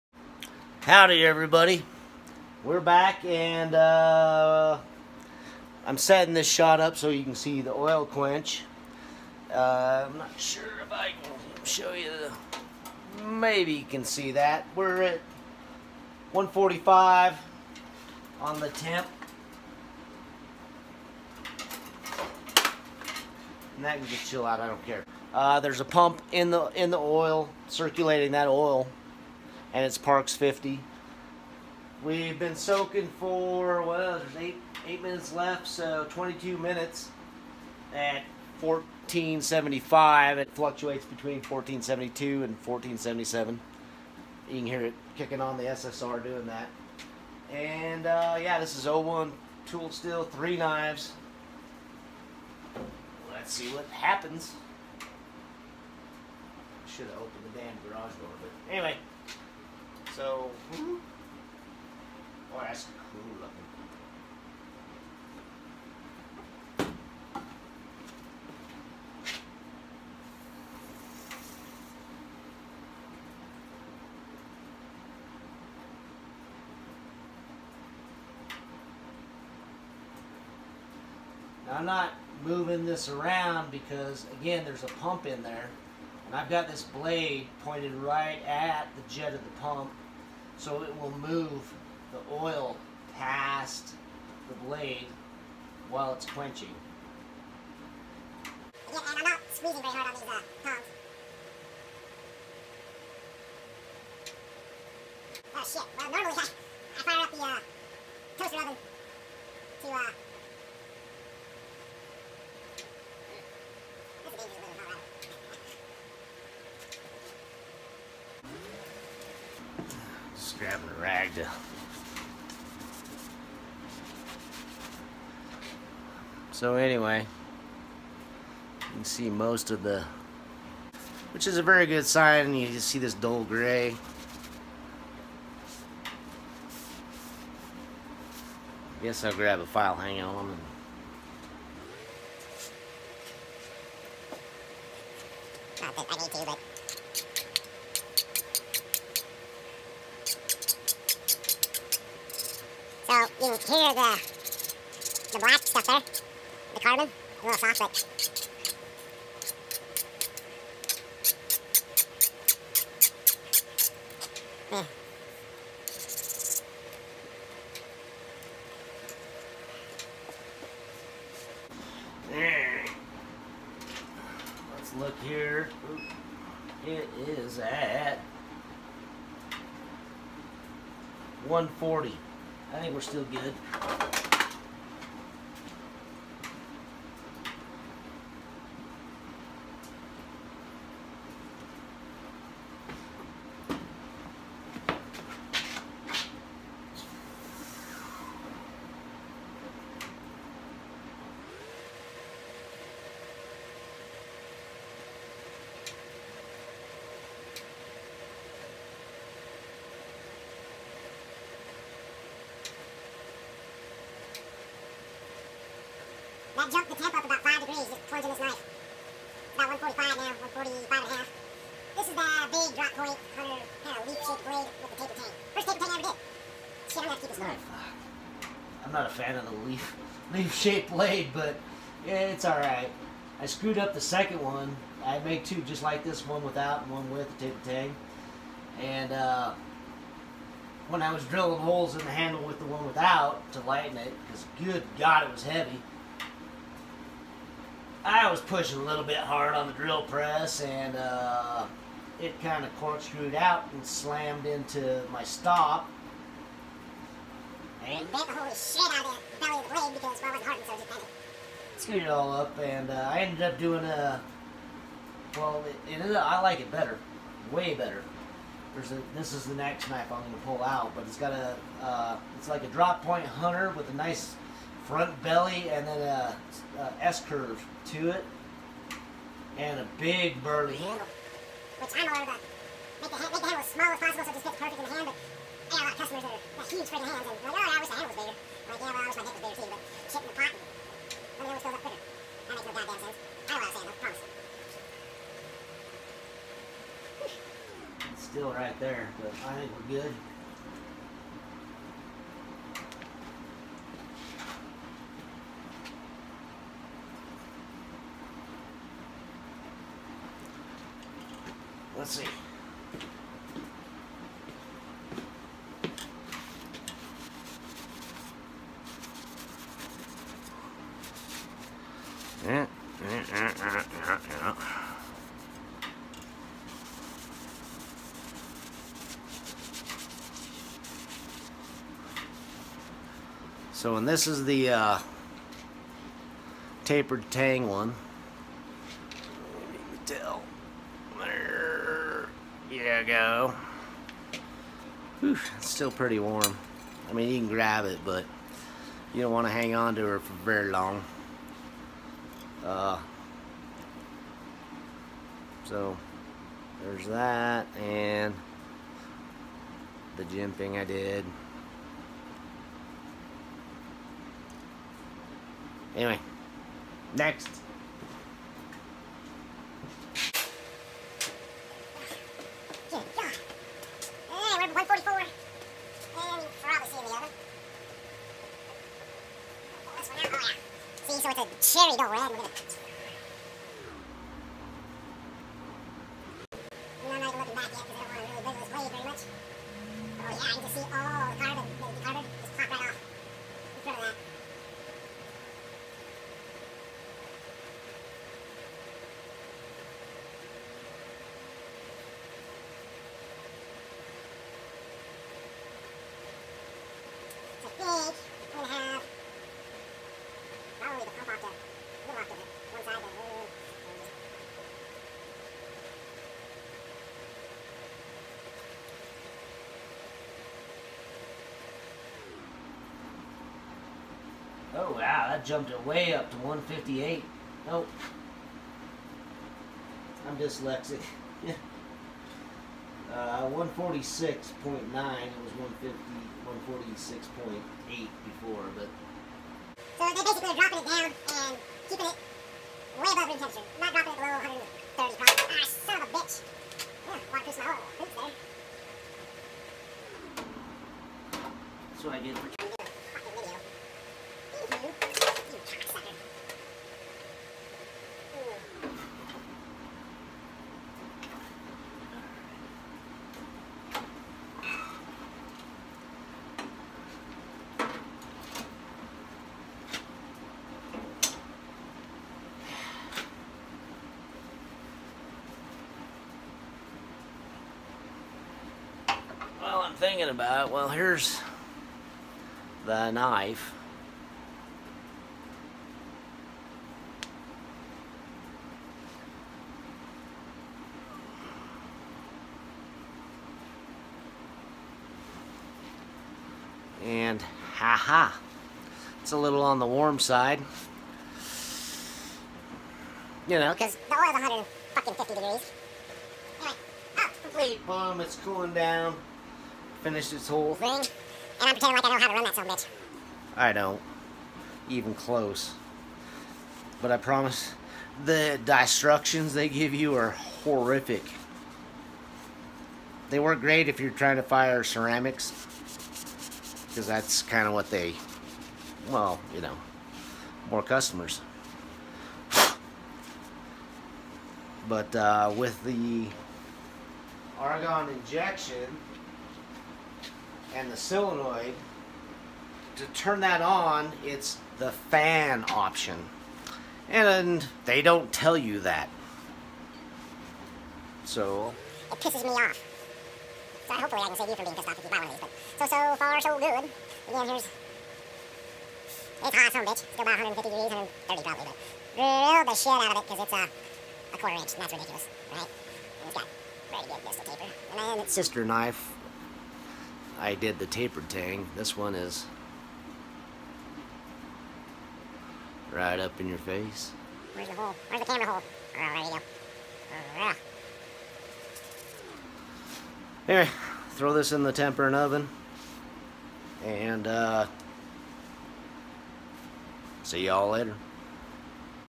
Heat treating 3 blades
Heat treat 3 blades O1 steel in parks 50 oil